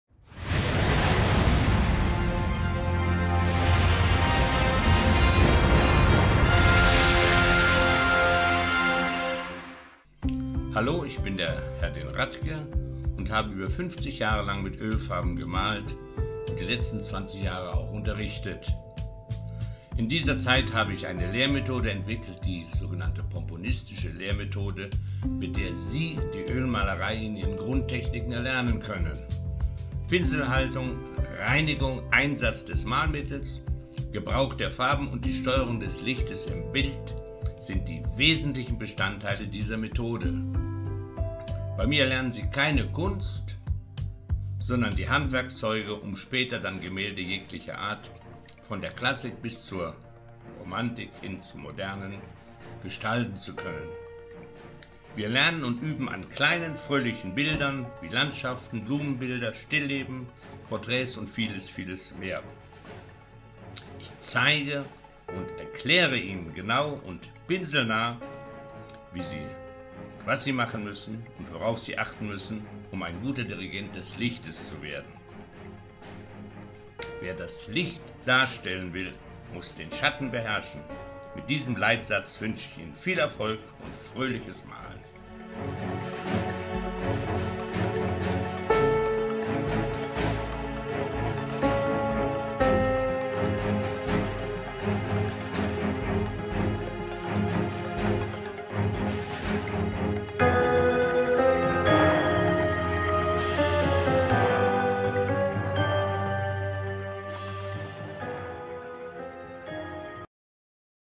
Trailer - Drei Rosen - Vorschau zum Workshop Öl- und Acrylmalerei
ATELIER HERDIN
Inhaltsübersicht zum Lehrfilm der zeigt wie man Rosen malt. Er eignet sich für Öl- und Acrylmalerei. Der Trailer zeigt ausgewählte Sequenzen in 4-facher Geschwindigkeit.